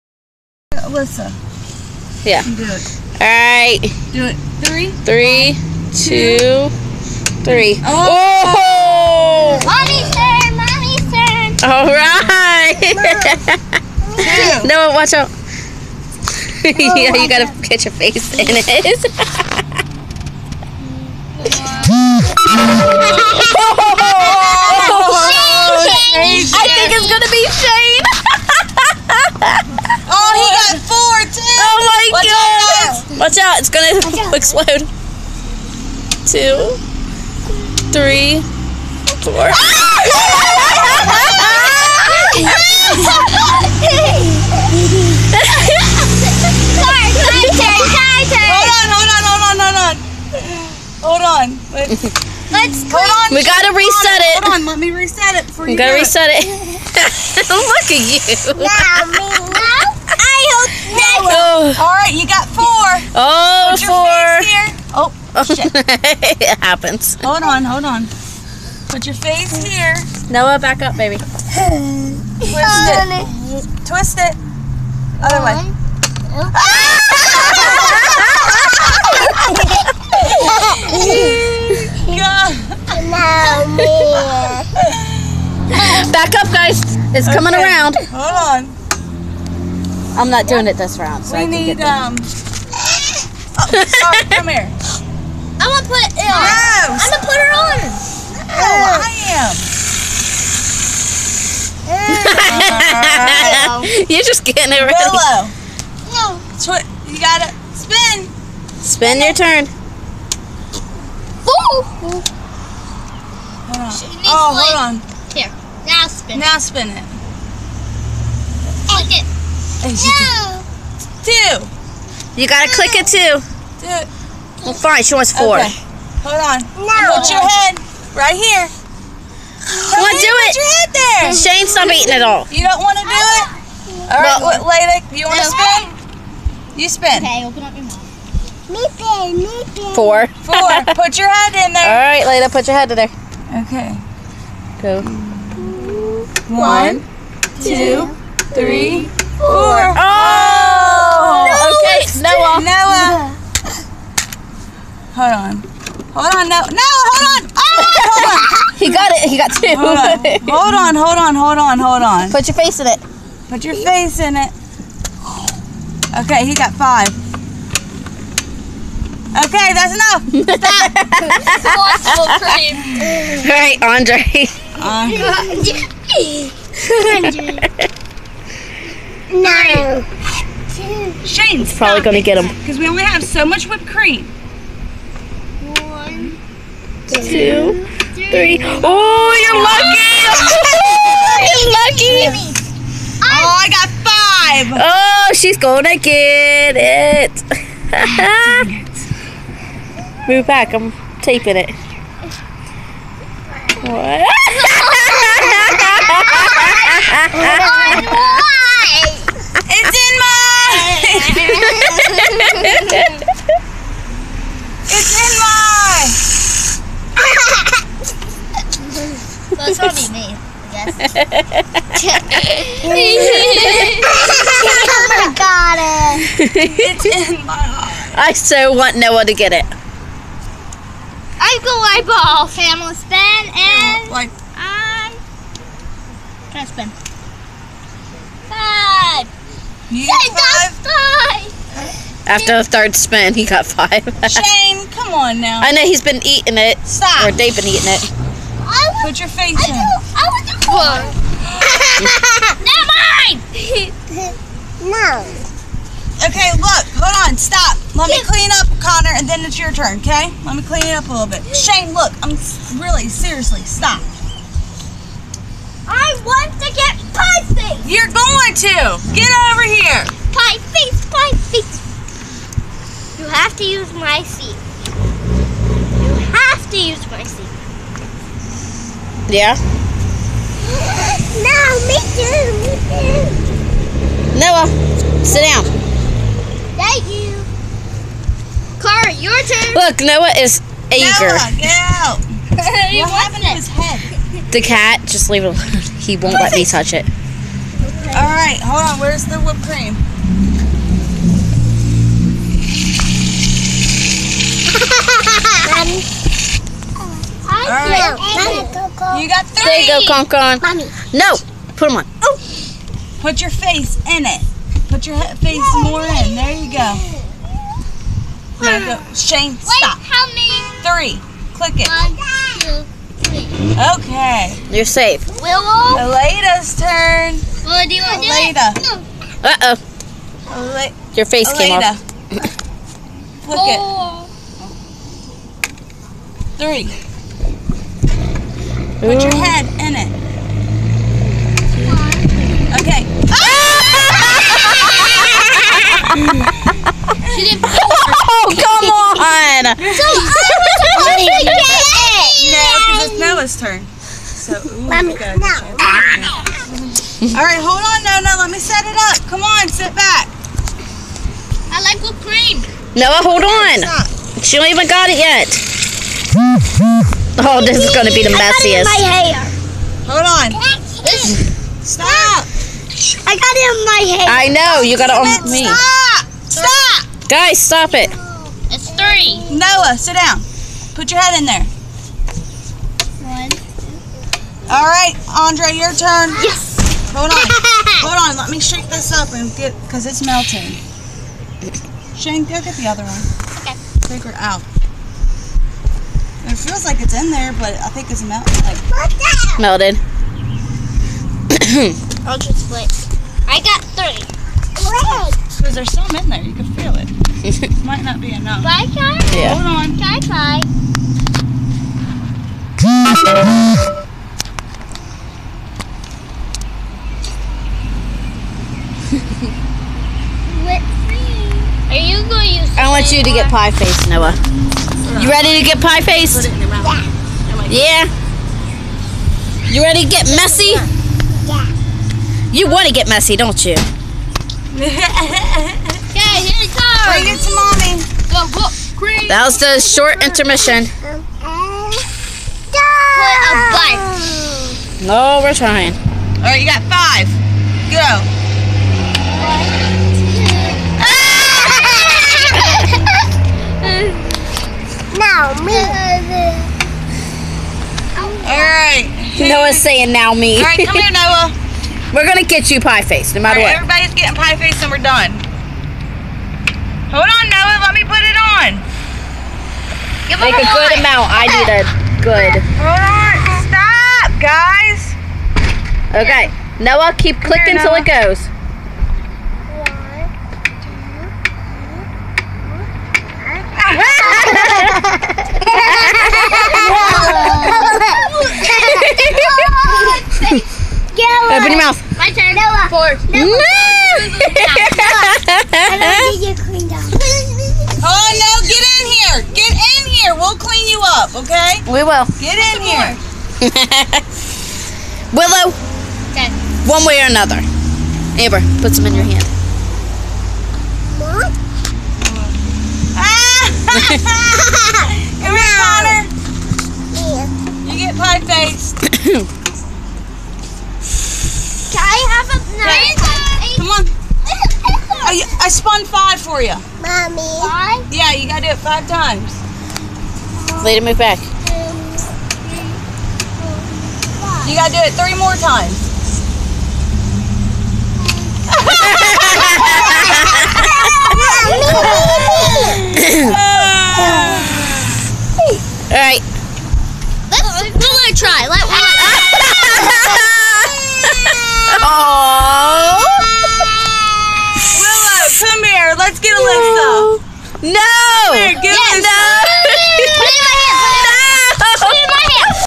Playing pie face
Fun with six kids